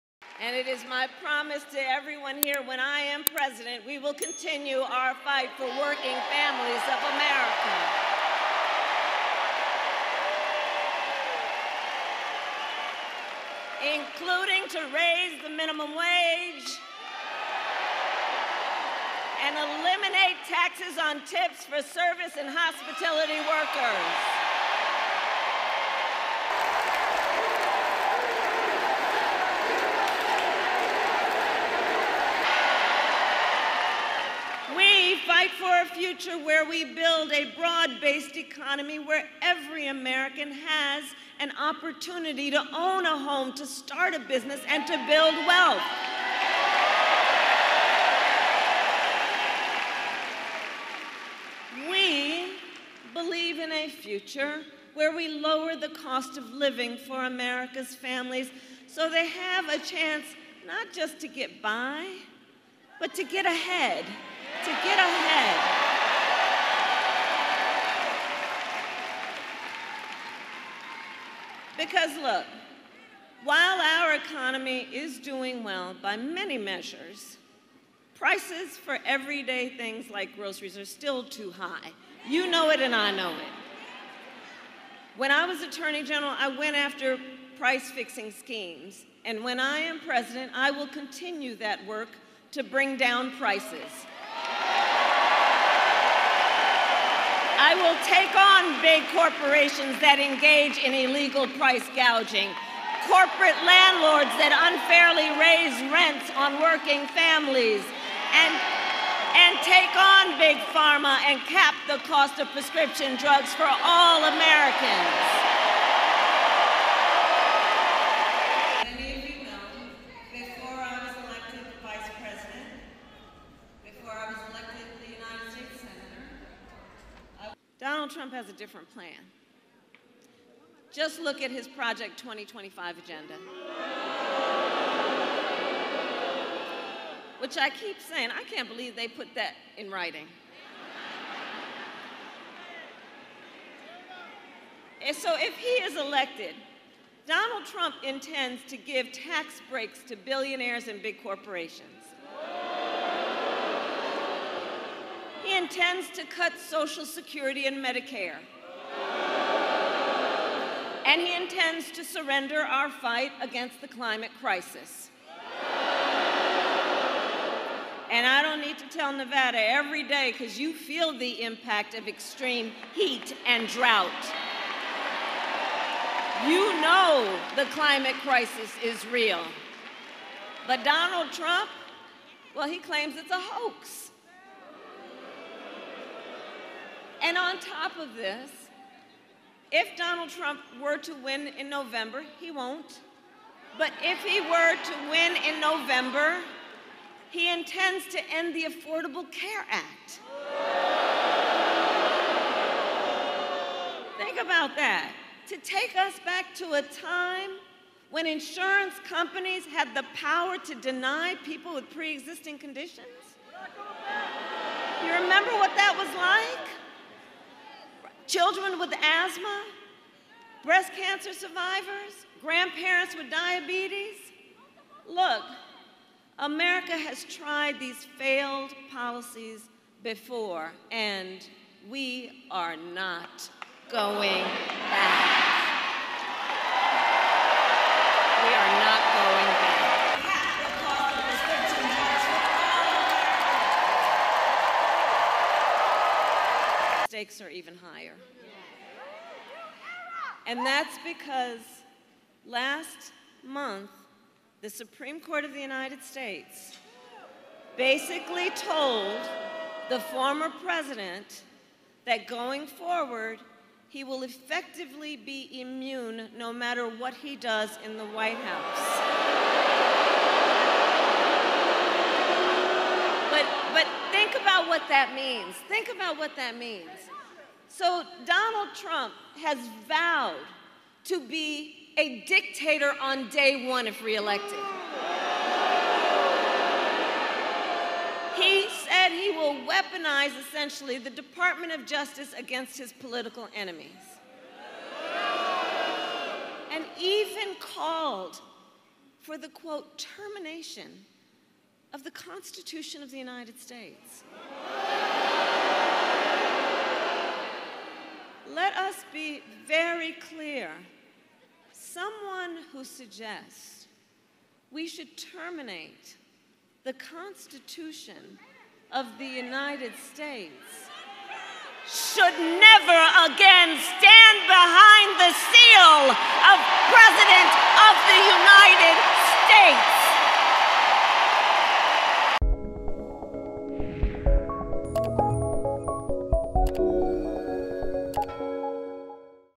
Kamala Harris Pledges to Eliminate Taxes on Tips | US Elections 2024 | The Express Tribune
Vice President Kamala Harris rallies in Nevada, promising to eliminate taxes on tips and battle big corporations to support working families.